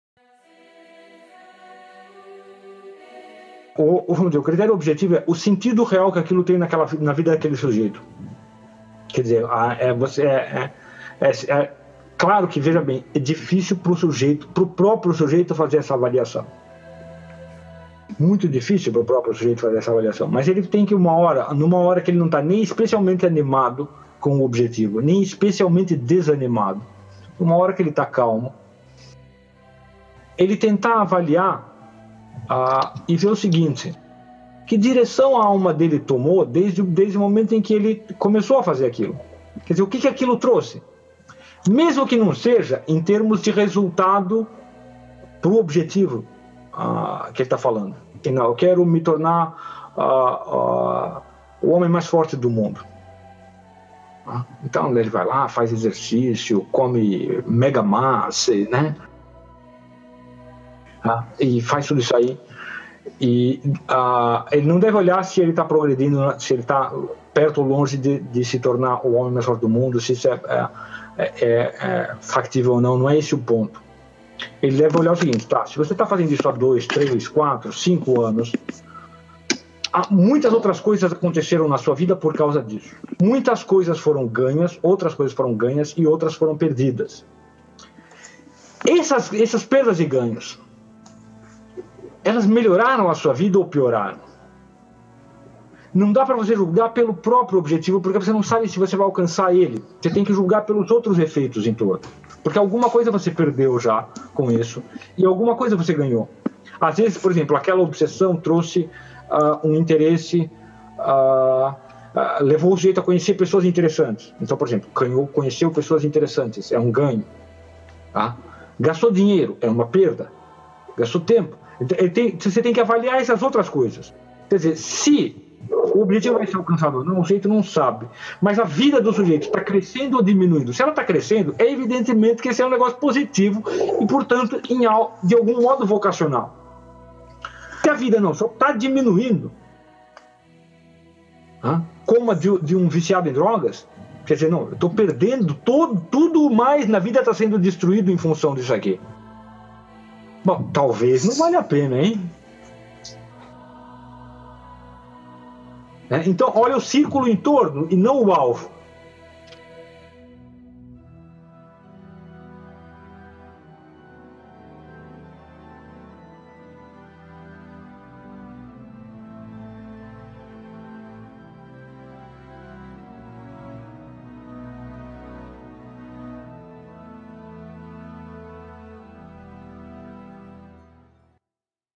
Obsessão vs Vocação - Luiz Gonzaga de Carvalho Neto
Muitas vezes o sujeito tem certas ideias, obsessões a cerca de tal trabalho, ou estudo, mas não sabe muito bem se aquilo é msm a sua vocação. Nesse trecho que eu recortei, Gugu nos tira essa dúvida de forma impar.

Edição:
Victor Gustavo